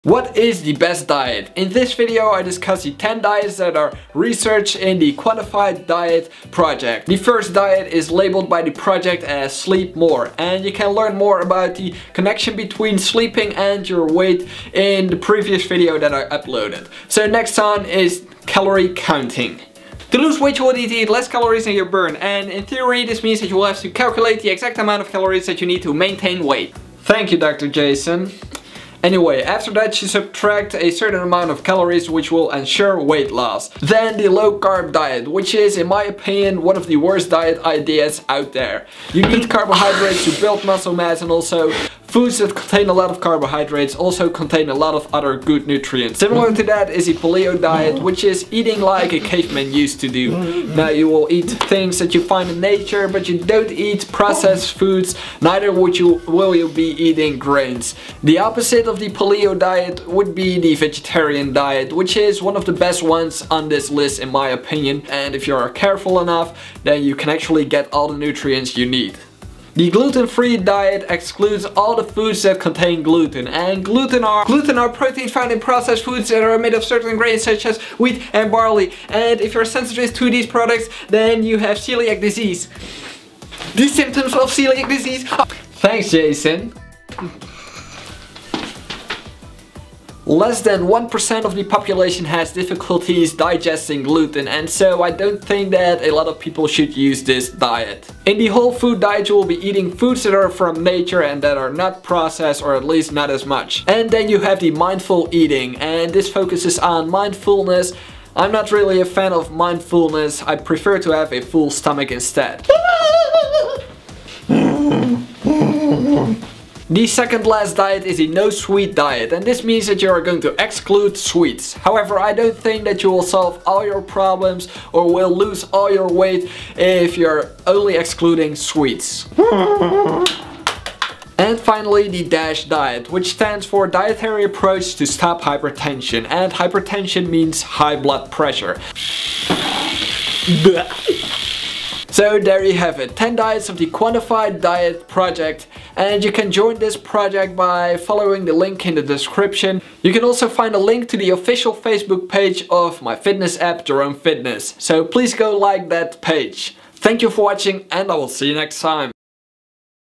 What Is The Best Diet?
I discuss 10 popular diets and help you to understand the use and uselessness of these diets. Dan Duet from 'Lift' asked me if I would help them promote a massive online study called 'the Quantified Diet Project'. As a big fan of science and research I decided to write a video script around the subject as I also wanted to cover more about nutrition. This is the result!

NOTE: I am not a supporter of any diet whatsoever. I prefer to achieve a healthy eating plan and lifestyle for my clients! The best way to lose weight is by a combination exercise and healthy eating in the right quantity.

what is paleo diet?
Low-carb diet bad?